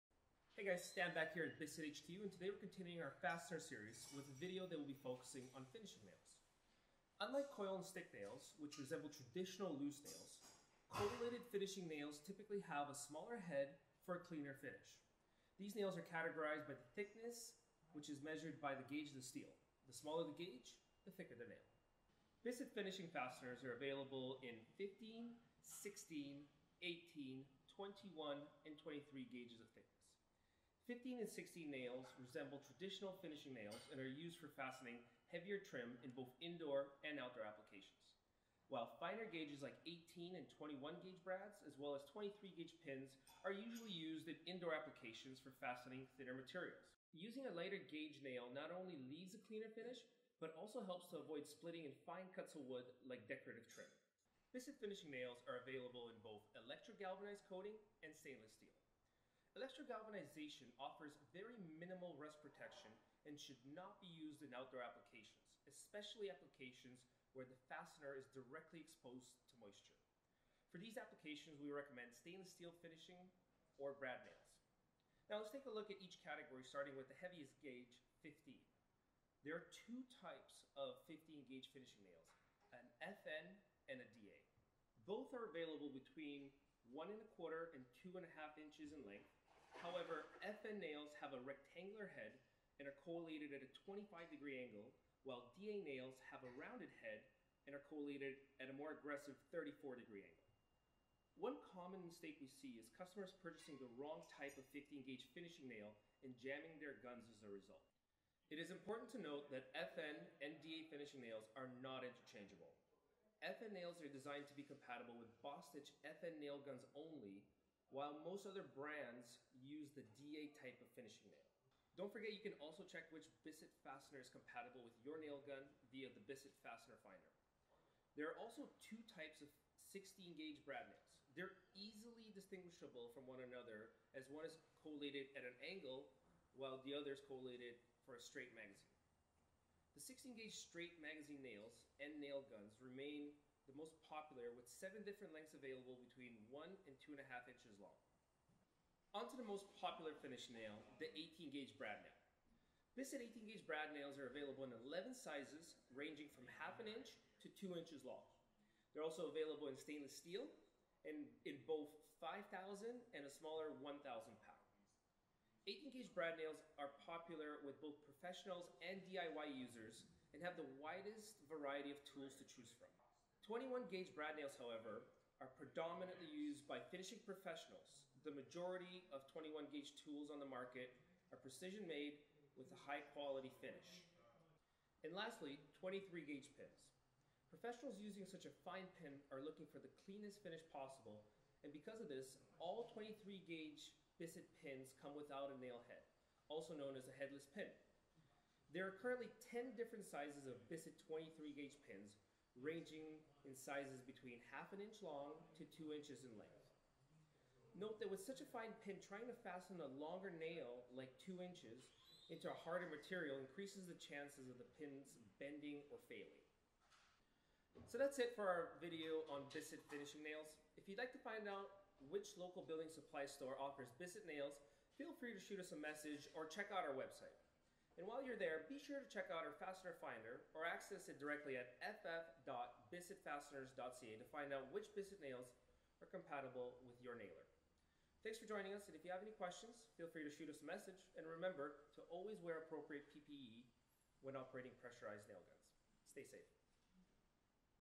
Bissett Fasteners - Finish Nails
Happy Monday everyone! Here is the latest video in our fasteners series focusing on finish nails 👍 Have any questions about today’s video?